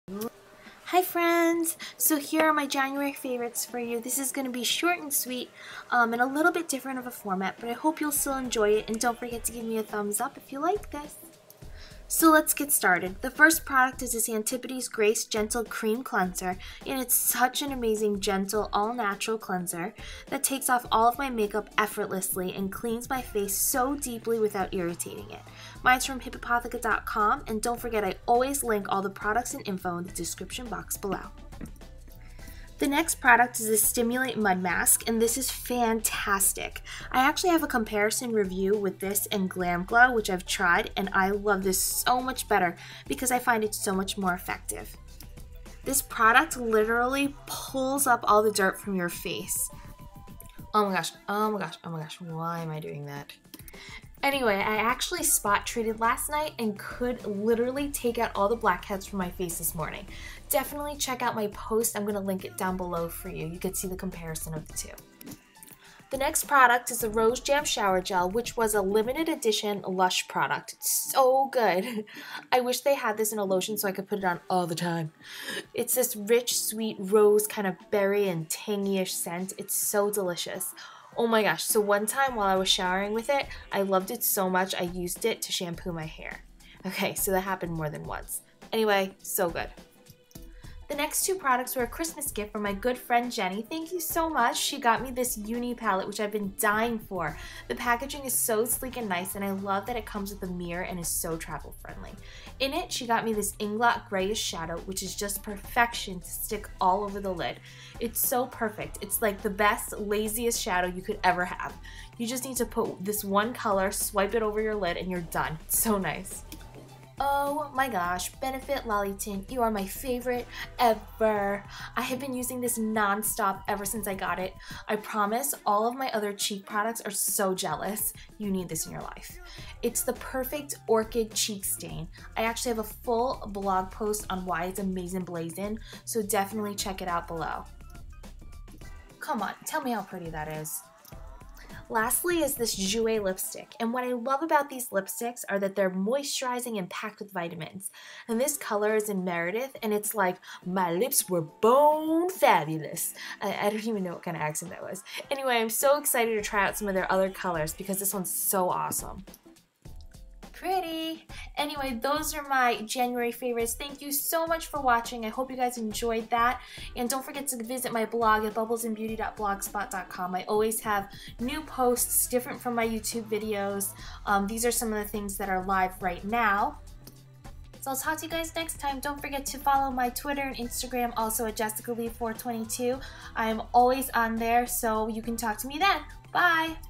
January Faves!!!
FIND ME!

THANK YOU SO MUCH JENNY for the gifts!!!

LUSH's Rose Jam (limited edition product)

Inglot Shadow in Matte 343: inglot.com

Earn CASH BACK when you shop!!! Ebates

ftc. antipodes cleanser and the stimul8 mud were sent to me for review but I had no obligation to say anything or even review them. I'm just simply sharing them because I lurve them so much... and hope you will too! :)